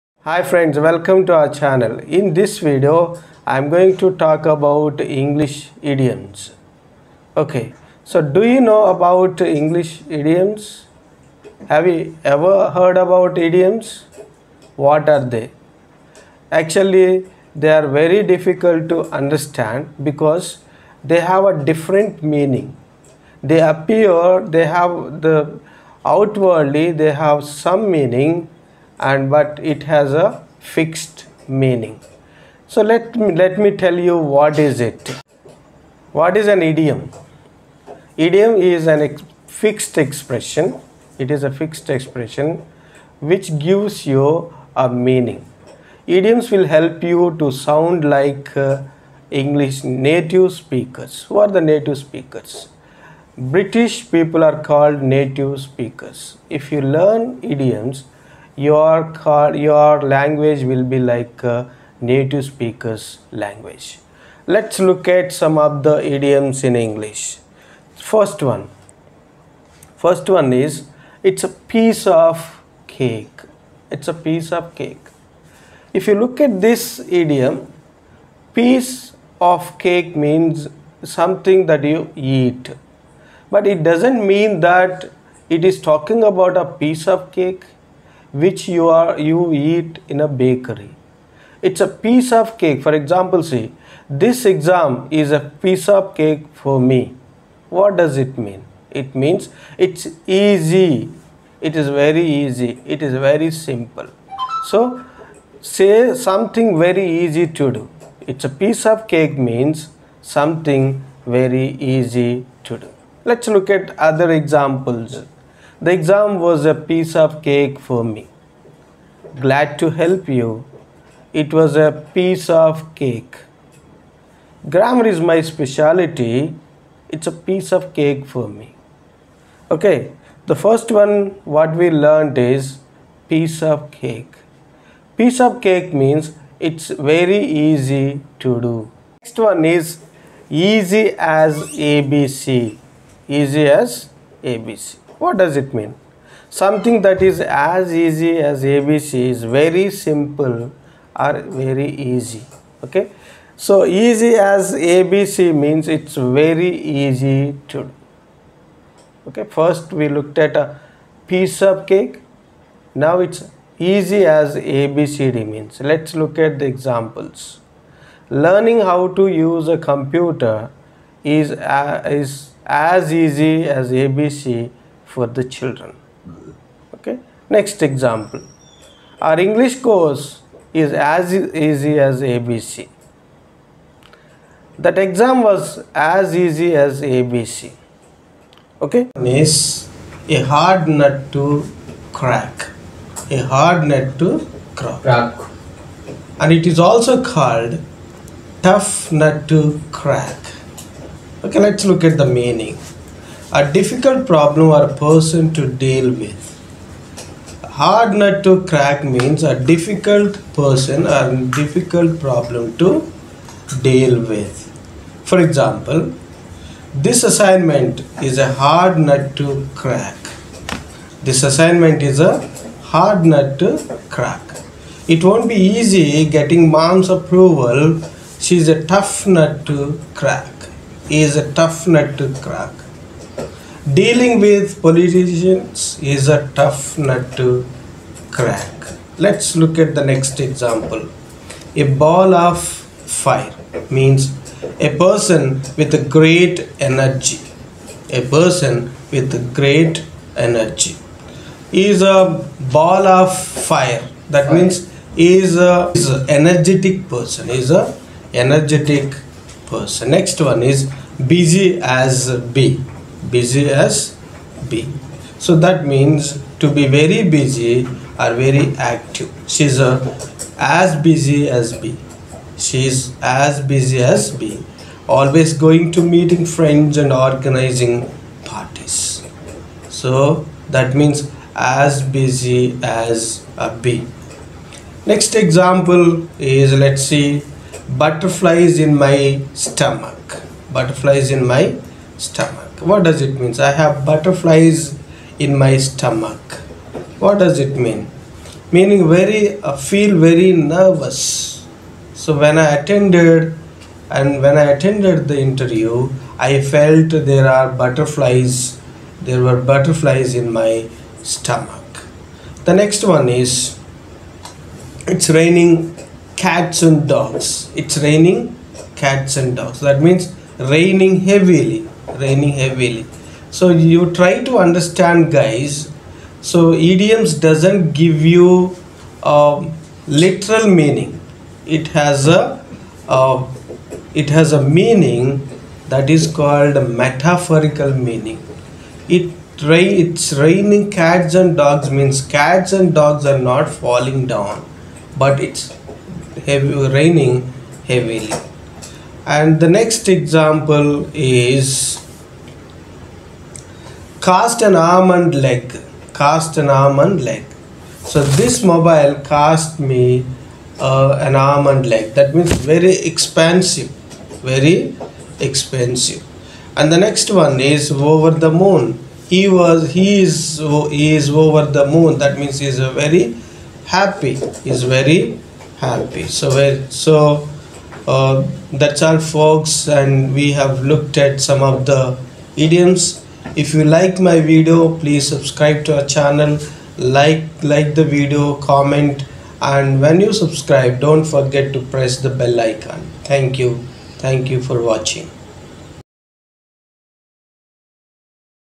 What is an Idiom?
An Idiom is a fixed expression which gives metaphorical meaning. it does not give us literal meaning.

raining cats and dogs
piece of cake
hard nut to crack